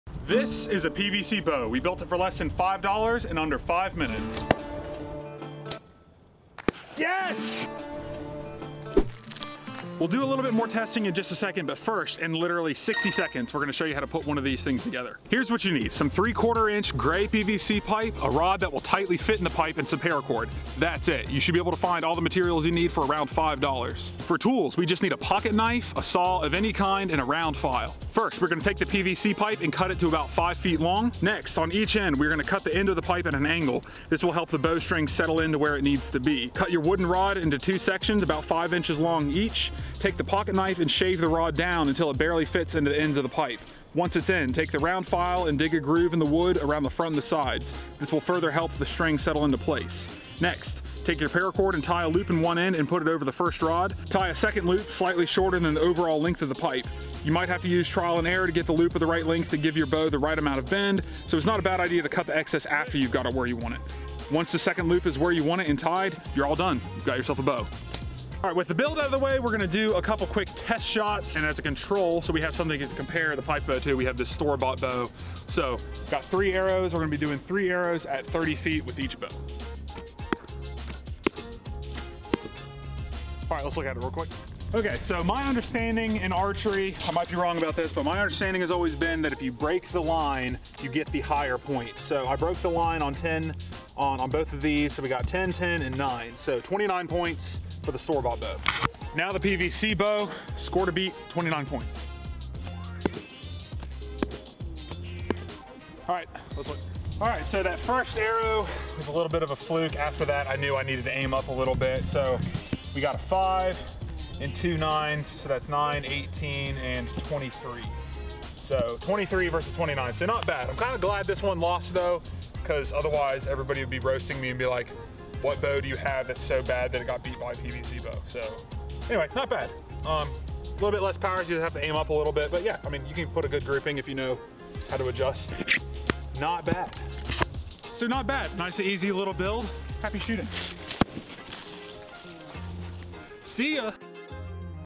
Build a $5 PVC Bow in 60 Seconds
I show you how to make a PVC bow in less than 60 seconds. These are super easy to make and can be done for about $5 in less than 5 minutes.